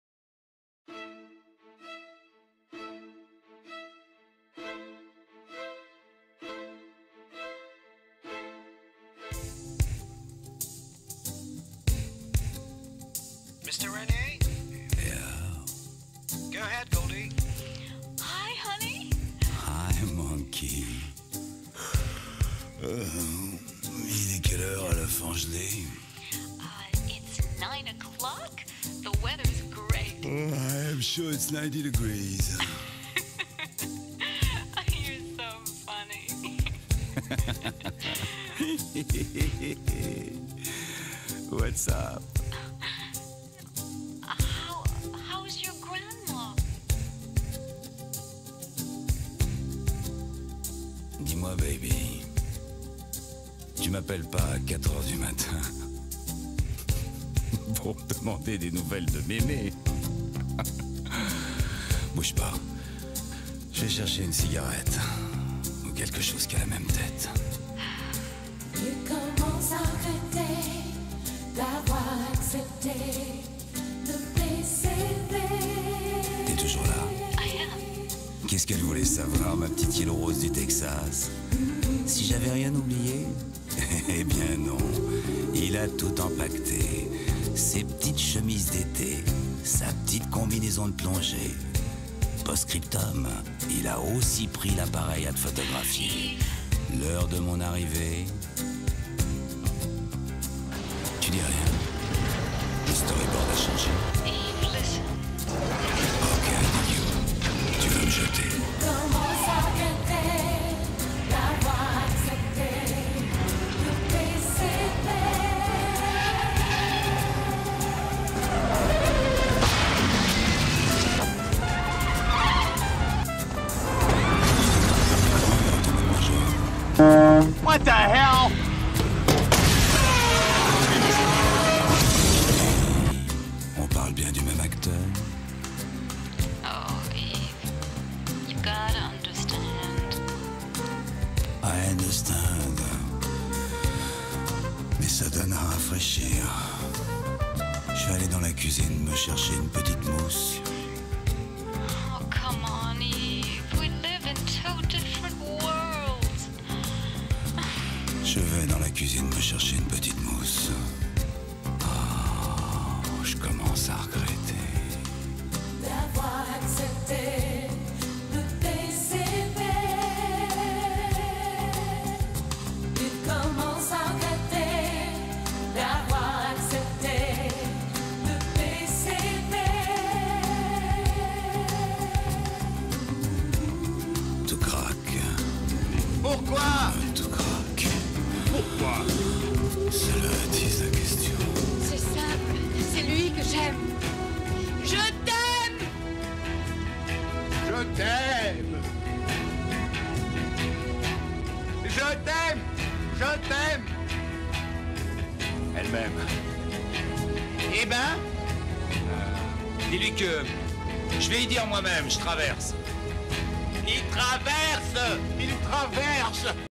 Yves et le PCV
Parodie et Hommage Yves Rénier Goldie Hawn GigaMusik Kurt Russell
Yves n'aurait jamais dû accepter le PCV